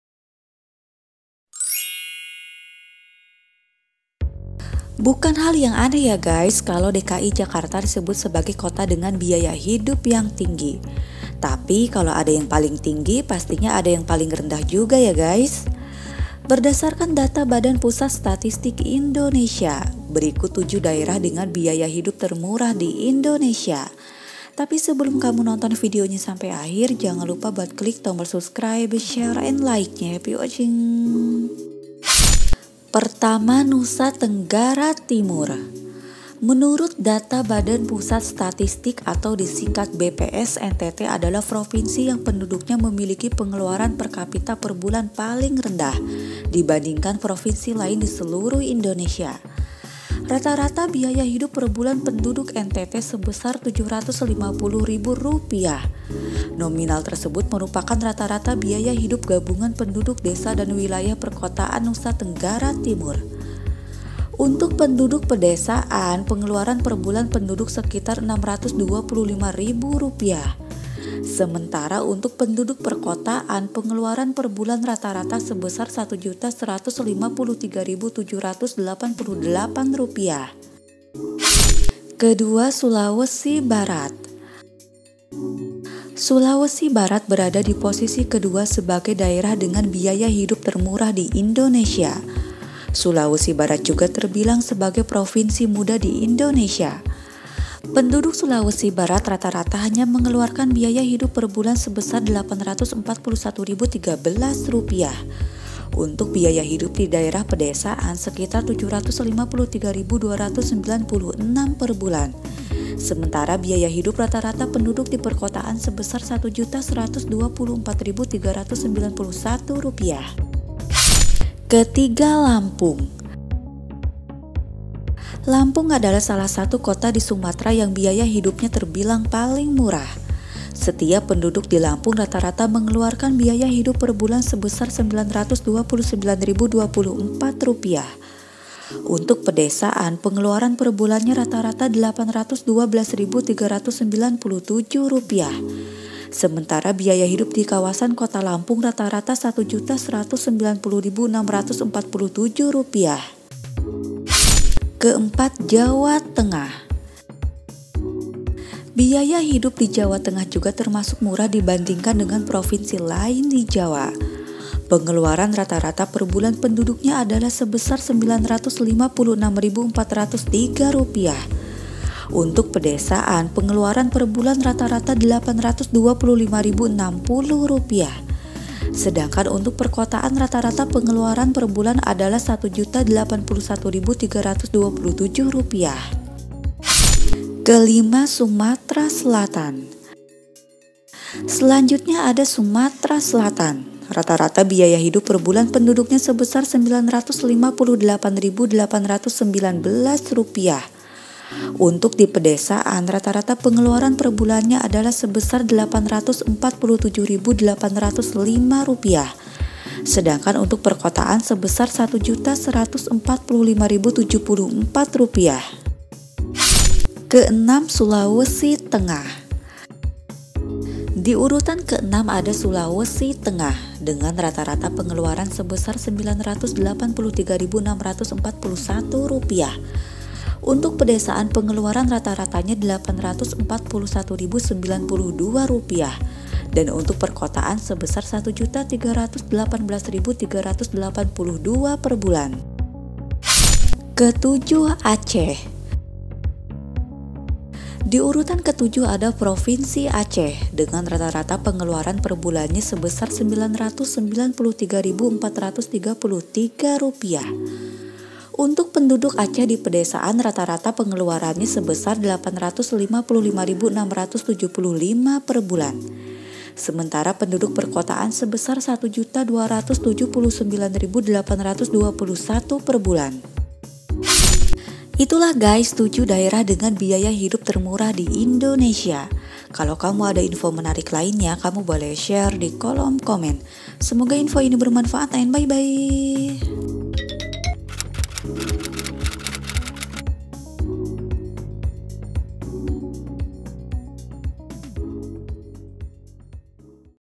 BIKIN PENGEN PINDAH! Inilah 7 Kota Dengan Biaya Hidup Termurah Di Indonesia
Bukan hal yang aneh ya gais kalau DKI Jakarta disebut sebagai kota dengan biaya hidup yang tinggi. Tapi kalau ada yang paling tinggi, pastinya ada yang paling rendah juga ya gais. Berdasarkan data Badan Pusat Statistik Indonesia.
Berikut 7 Kota dengan biaya hidup termurah di Indonesia.

  CHANNEL : Andy Quan